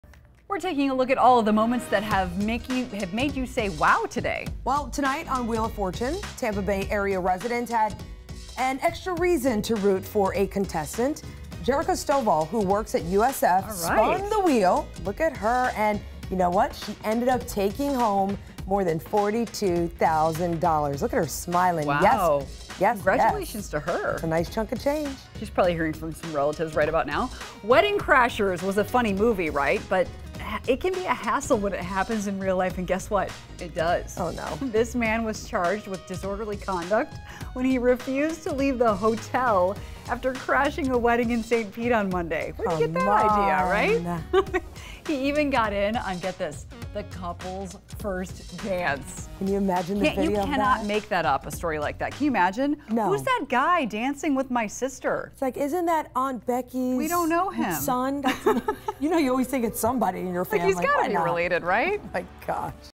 Wow Moments: St. Pete woman is lucky on 'Wheel of Fortune'
Jerrica Stovall won thousands of dollars on the popular game show. Also, an uninvited wedding is arrested after he jumped into the couple's first dance.